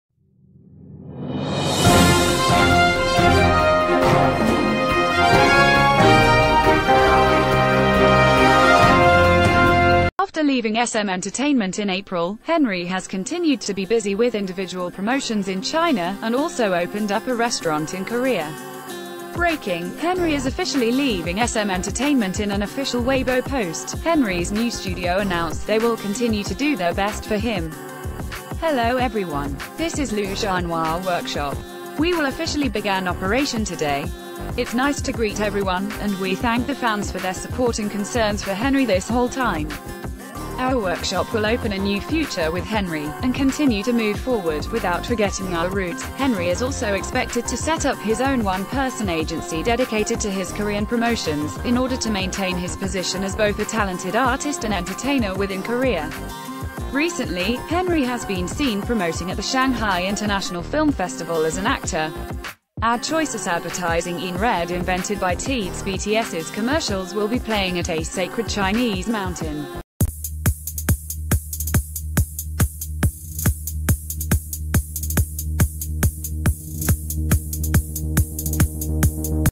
Henry Set Up Personal Worskhop In China, Plans On Creating 1-Person Agency In Korea
After leaving SM Entertainment in April, Henry has continued to be busy with individual promotions i...